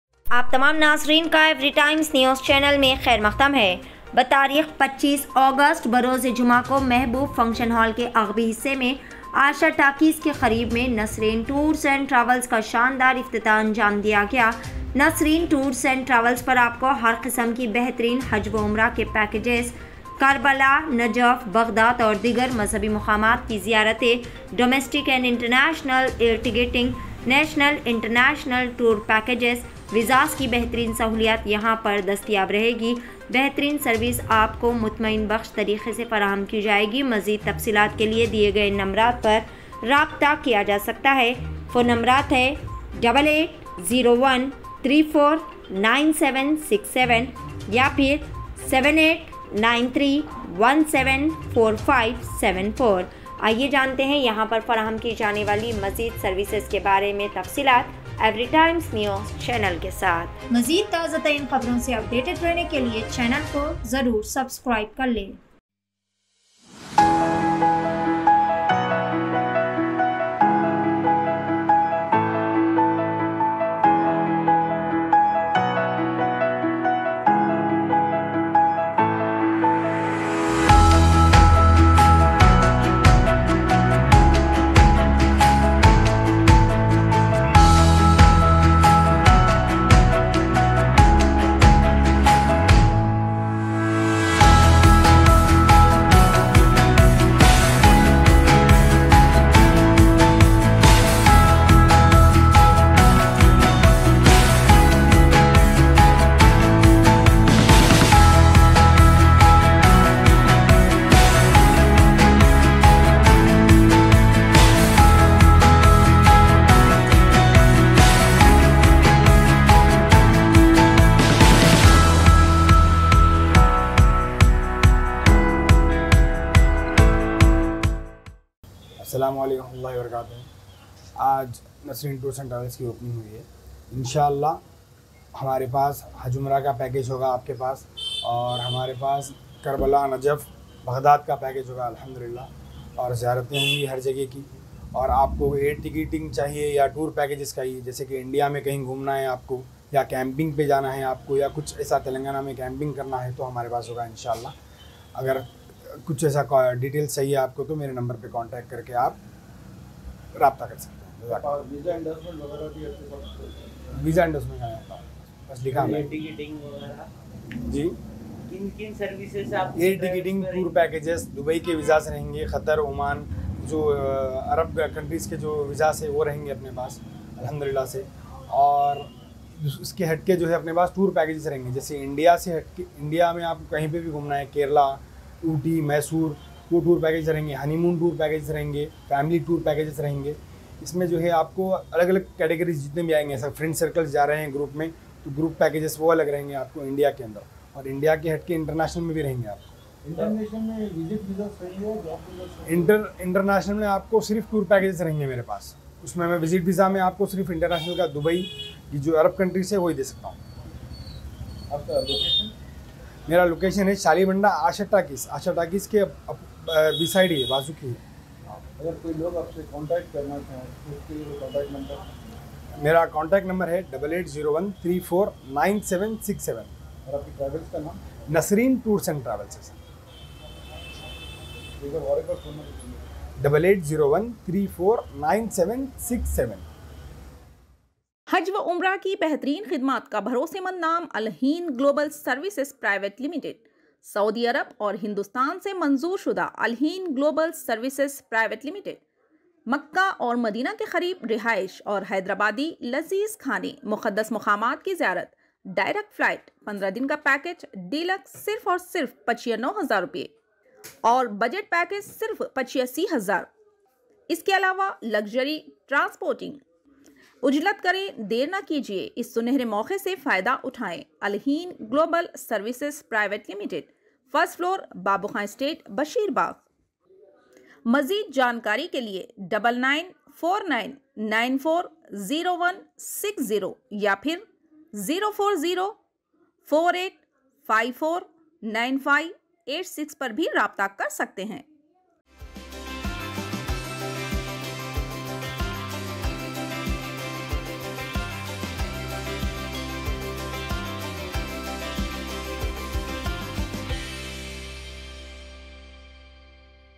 Start Your Trip With Nasreen Tours And Travels | Opening Ceremony | Shahali Banda | Hyderabad...
Start Your Trip With Nasreen Tours And Travels | Opening Ceremony | Shahali Banda | Hyderabad |@Etnewshyderabad

FOR DETAILS:-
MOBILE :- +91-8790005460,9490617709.